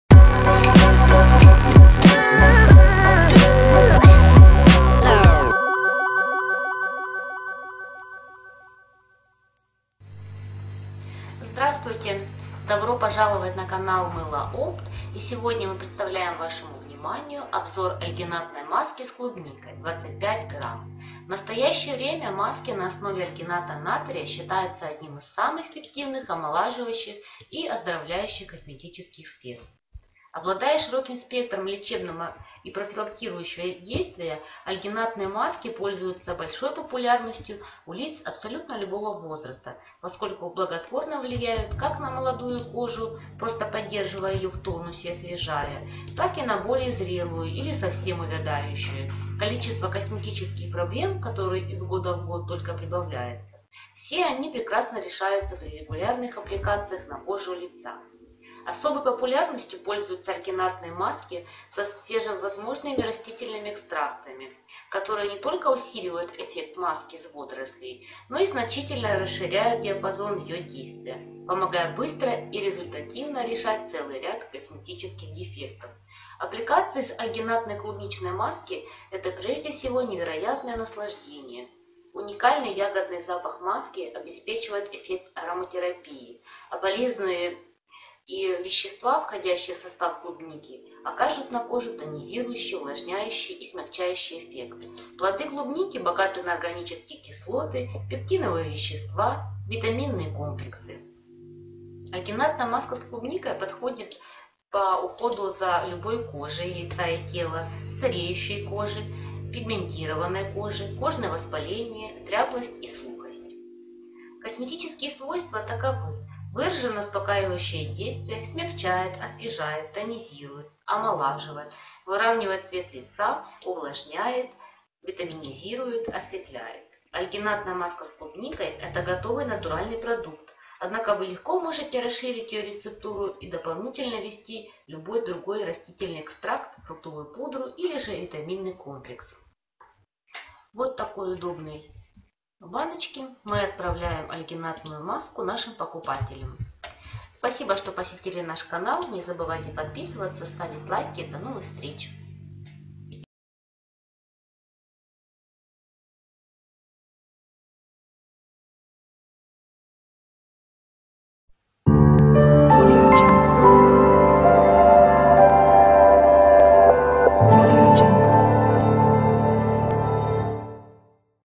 Альгинатная маска с клубникой // Маски для лица // Альгинат натрия в косметике
Видео обзор Альгинатная маска с клубникой от магазина Мыло-опт